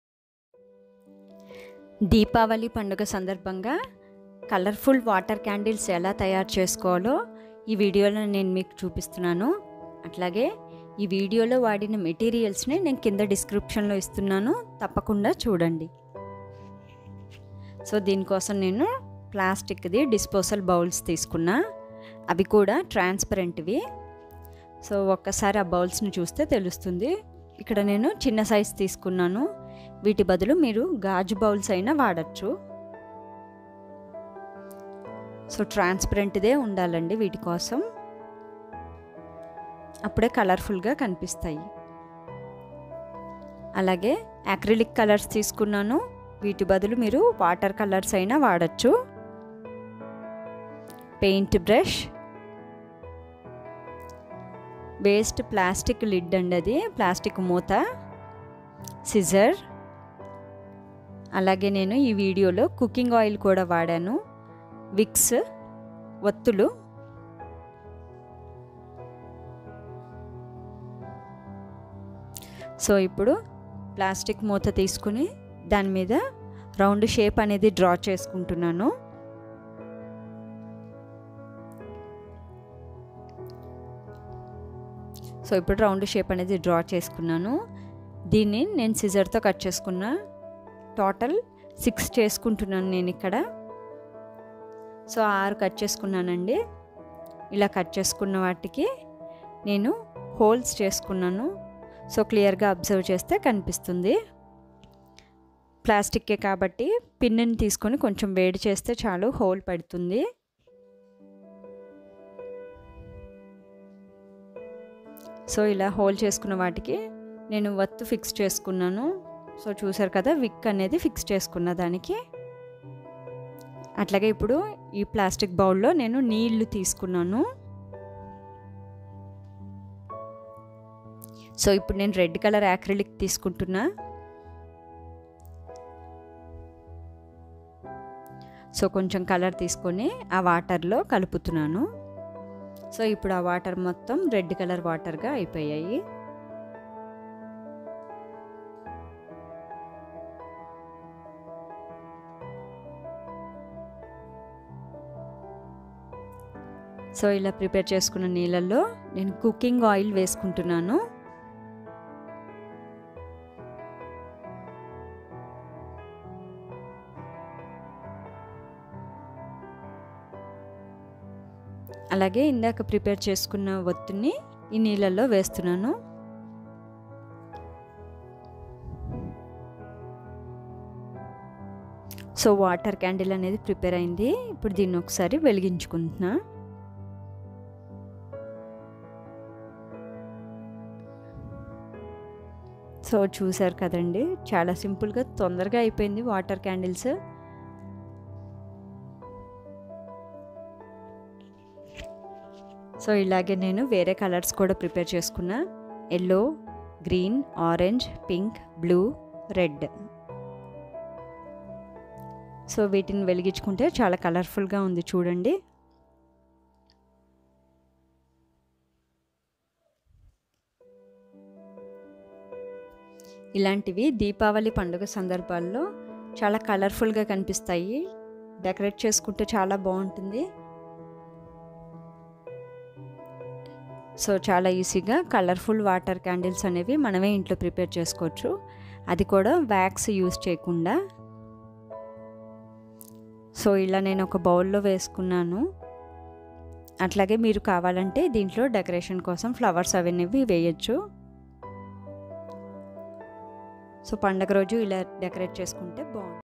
Diwali special colourful water candles making at home//diy water candles//water candles ideas//
materials required : 1. disposal transparent bowls 2. wicks 3. water colours or acrylic paints 4. water 5. cooking oil 6. paint brush for mixing 7. plastic lid 8. scissor